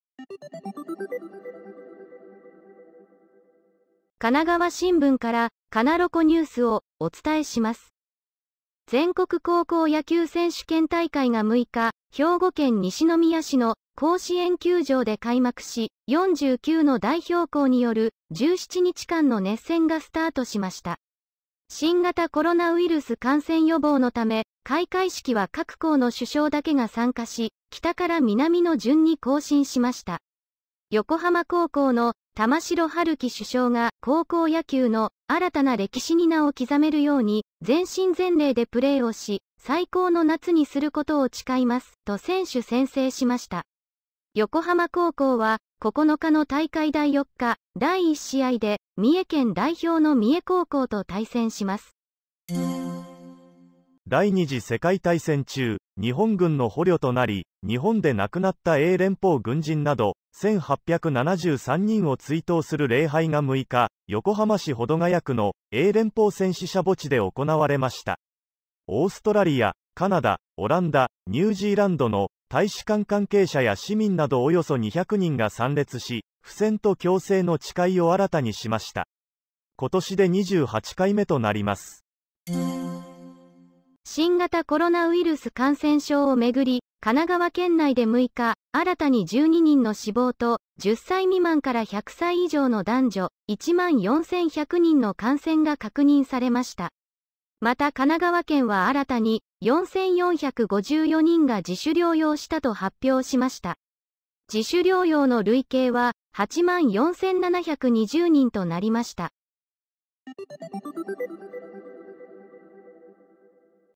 甲子園が開幕、横浜主将が宣誓 AIアナ・８月７日／神奈川新聞（カナロコ）
神奈川新聞から「カナロコニュース」をお伝えします。
・甲子園が開幕、横浜主将が宣誓（神奈川県）
・横浜・英連邦戦死者墓地で礼拝（横浜市）
・１２人死亡、１万４１００人感染（神奈川県）